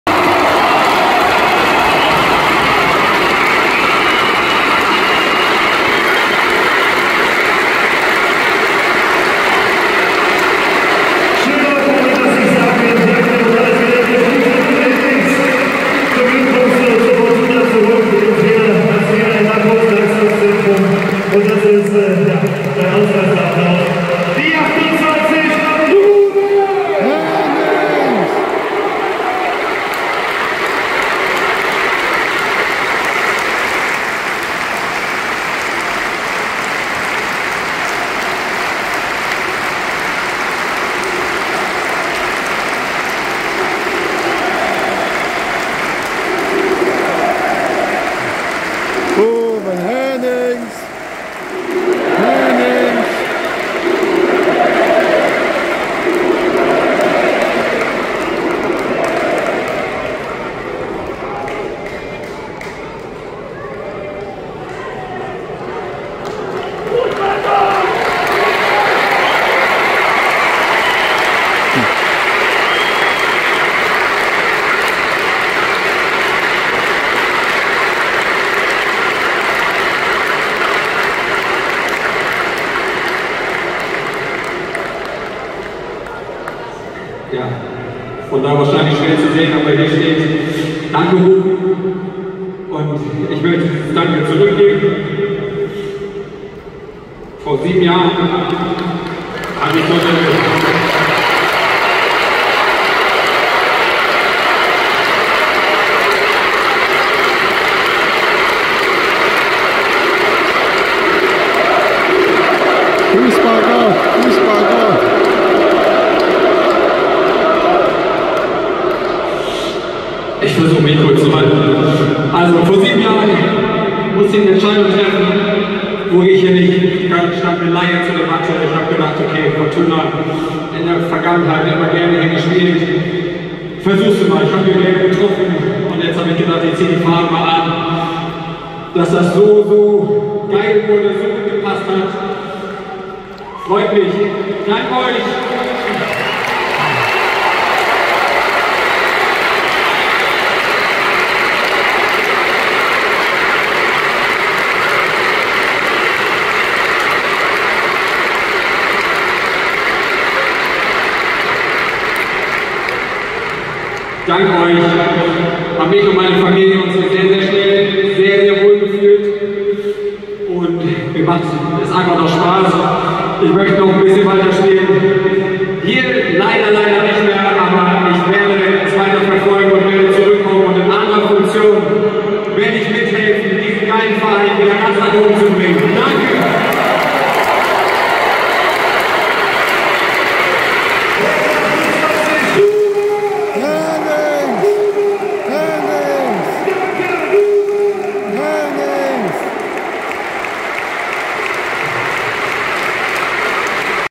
Fortuna Düsseldorf vs Hannover 96 21.05.23 Rouwen Hennings Verabschiedet sich von den Fans Teil2😊🥲😎
Wenn dir mein Video gefallen hat🙂, dann:
Lass ein Like da sowie ein Abo auf meinen Kanal 😃😎
Und Aktivier die Glocke😊
Würde mich darüber Freuen😍
Du kannst gern auch das Video Kommentieren😎